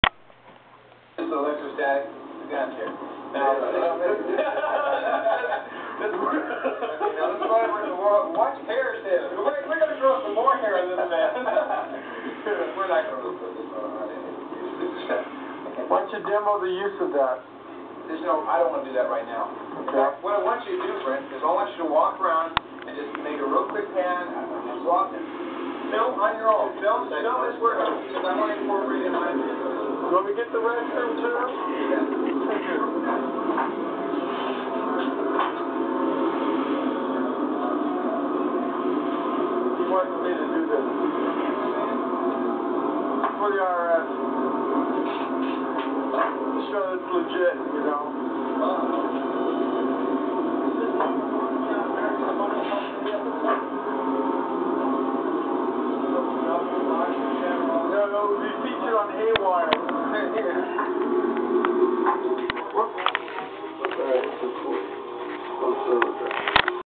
a visit to fitzgeralds of yesterday
dan and Lee and camera surprise dean,John and Jim.